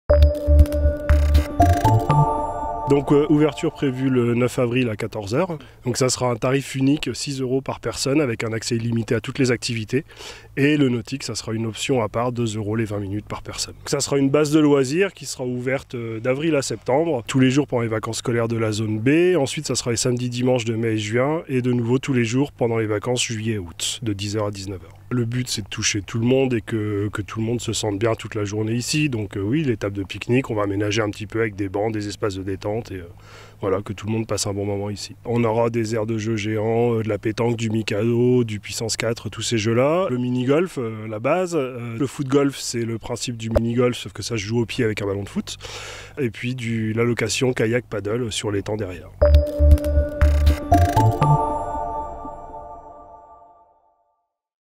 🚣‍♀️ LA BASE DE L'ETANG OUVRE SES PORTES LE 09 AVRIL 2022 🚣
C'est tout nouveau dans le département, une base de loisir ouvre ses portes le 09 avril 2022 avec comme activité :

⚽️Foot-golf
🏐Pétanque
🧗‍♂️Parcours acrobatique
⛳️Mini-golf
🏄Paddle
🚣‍♀️Canoé

L'entrée est unique (sauf pour le paddle, canoé et autre activité aquatique) de 6 euros seulement ! 😍

Un excellent moyen de passer du bon temps en famille avec beaucoup d'activités !🥳

Nous vous encourageons tous à découvrir cette base de loisir qui ne demande qu'à grandir ! 🤩

➤➤➤ Sources en fin de description

📚VOUS POUVEZ RETROUVER NOS DIFFERENTES EMISSIONS EN SUIVANT LES LIENS 📚

Nous remercions tous les intervenants directs ou indirects dans l'élaboration de la vidéo !